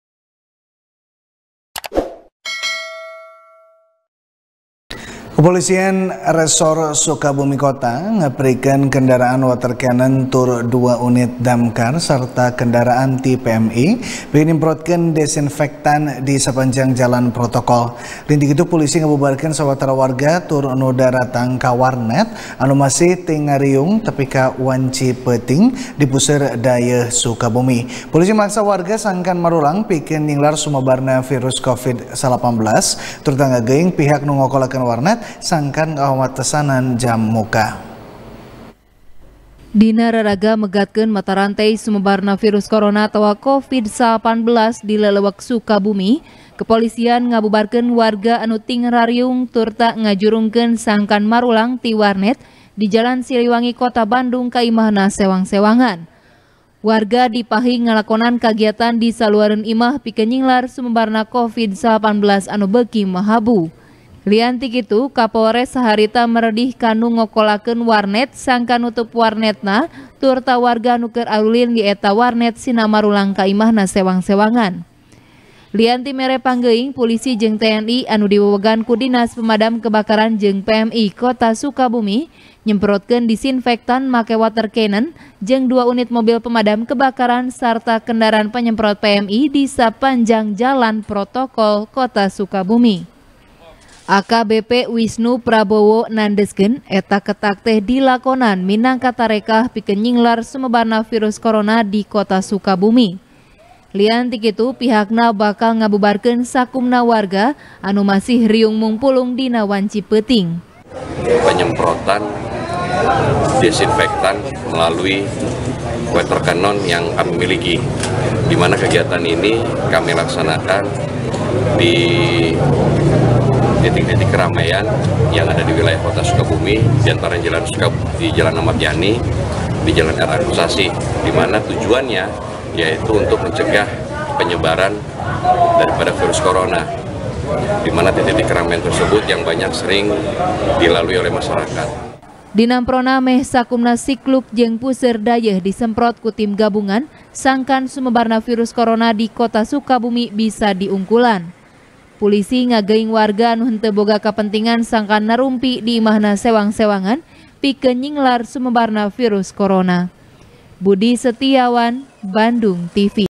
Nyinglar Covid-19 Pulisi Tur Tni Nyemprotkeun Disinfektan Wanci Peuting | TP | BANDUNGTV
TP | BANDUNGTV | DERI FIRMAN | Kepolisian Resor Sukabumi Kota, Ngeprikeun Kendaraan Water Canon Tur Dua Unit Damkar Sarta Kedanraan Ti Pmi, Pikeun Nyemprotkeun Disinfektan Di Sapanjang Jalan Protokol. Lian Ti Kitu, Pulisi Ngabubarkeun Sawarata Warga Tur Nu Daratang Ka Warnet Anu Masih Tingrariung Tepi Ka Wanci Peuting, Di Puseur Dayeuh Sukabumi. Pulisi Maksa Warga Sangkan Marulang Pikeun Nyinglar Sumebarna Virus Covid-19 Turta Ngageuing Pihak Nu Ngokolakeun Warnet Sangkan Ngawatesan Jam Buka.